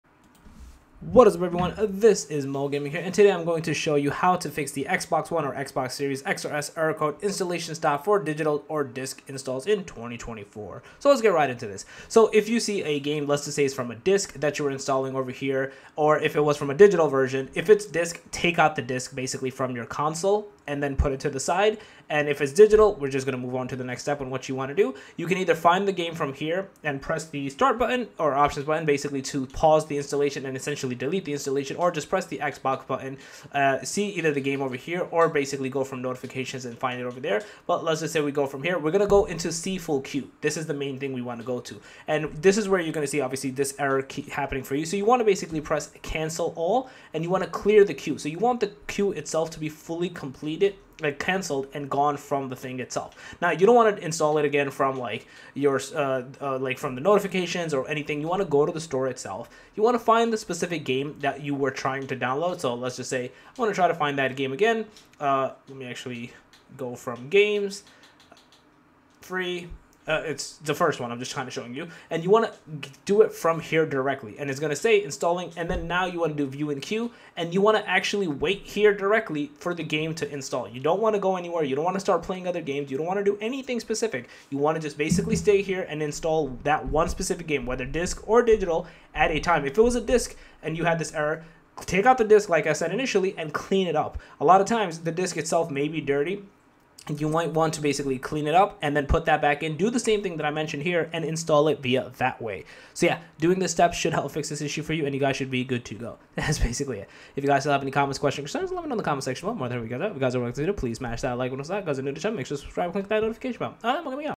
How To Fix Xbox One / Series X/S Error "Installation Stopped For Digital Or Disc Installs" - 2024
In this video I talk about How To Fix Xbox One / Series X/S Error "Installation Stopped For Digital Or Disc Installs" - 2024